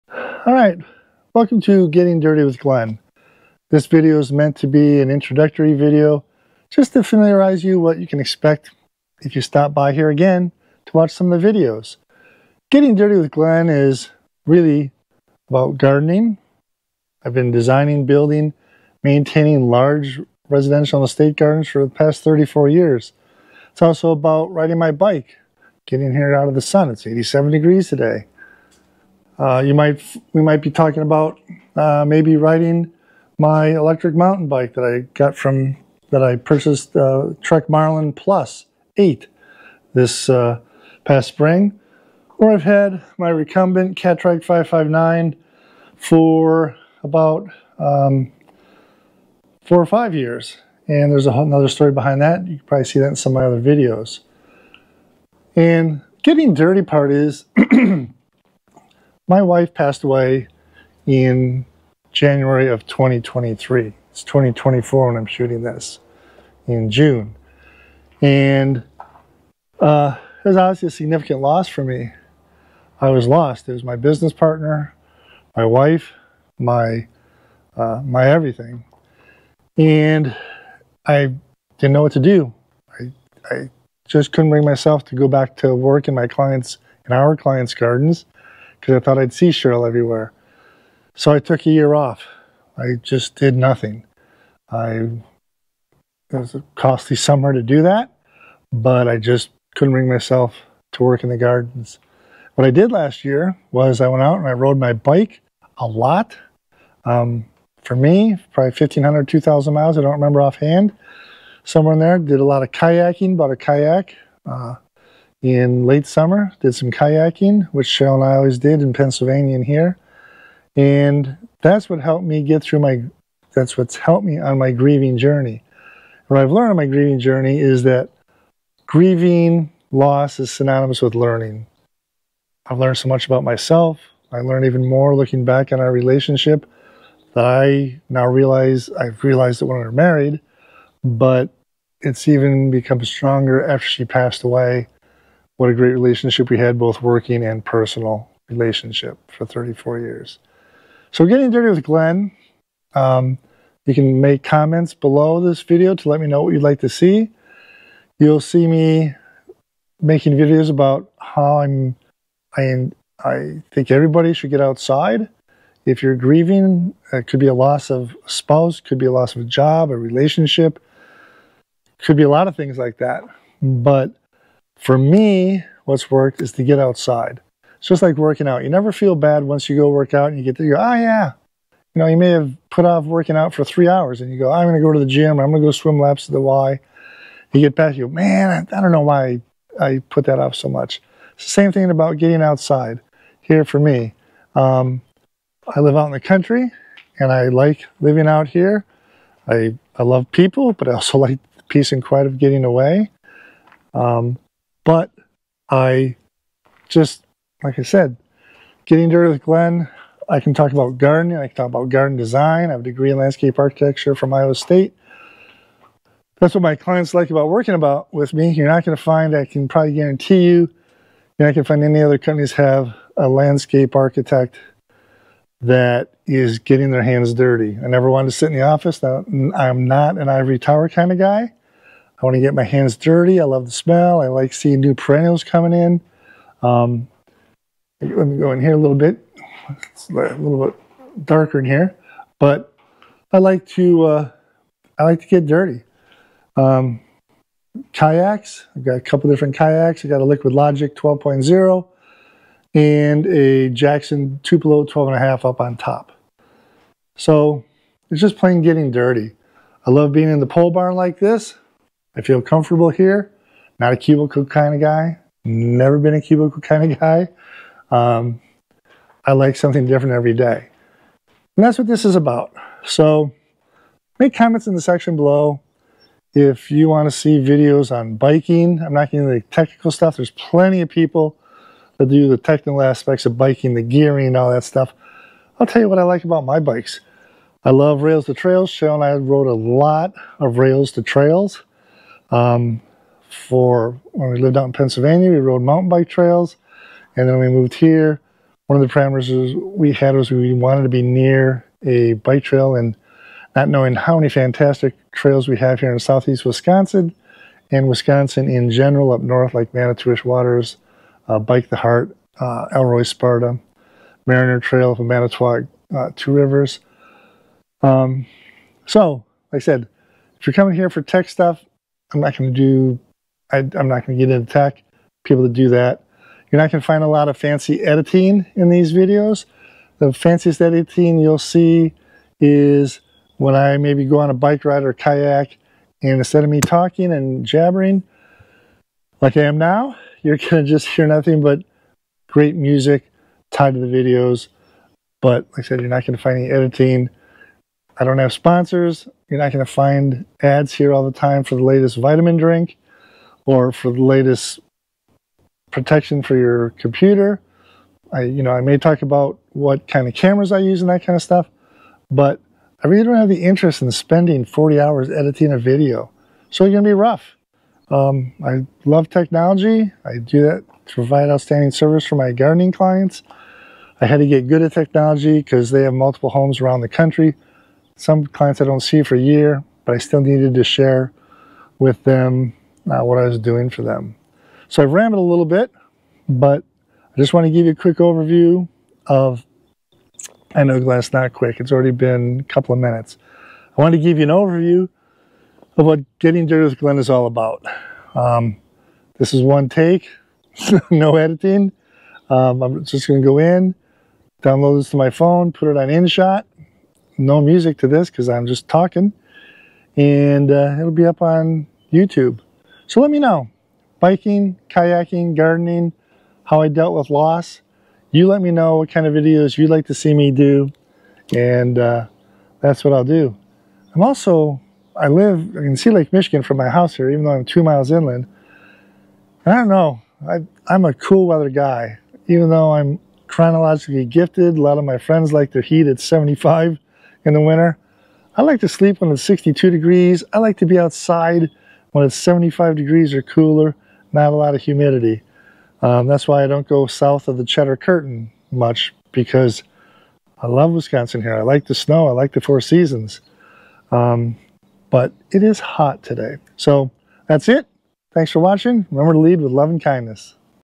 Are you ready to get dirty?
Welcome to Getting Dirty with Glenn.  In this video I share with you the who, what and where of Getting Dirty.  I will share my 45 years of experience, designing, building and maintaining large residential and estate gardens along with my passions of cycling, kayaking and simply getting outdoors.

When my wife of 34 years , Cheryl, passed away in January of 2023 I truly have embraced everything we loved about the outdoors to help my on my journey of grieving, loss and moving forward with my new life.

Remember to Lead with Love and Kindness